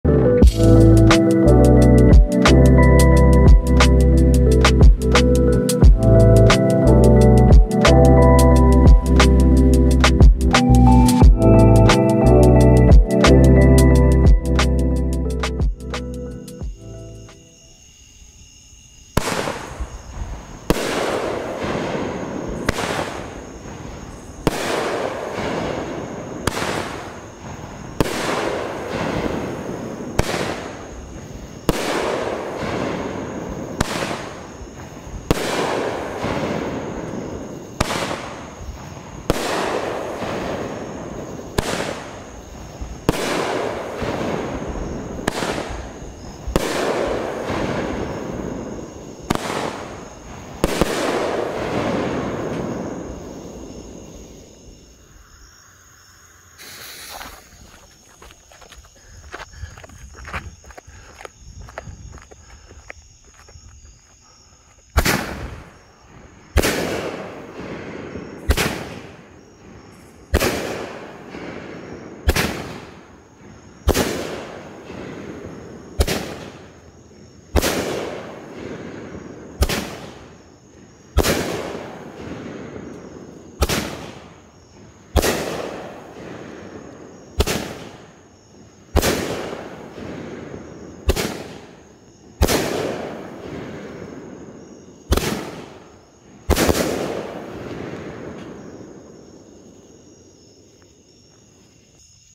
Emera - Smoking Alien - 10 Shot 500G Firework (TWO VIEWS)
First look at Emera by Smoking Alien. 10 shots of color dahlias & a 2-shot finale. Solid cake!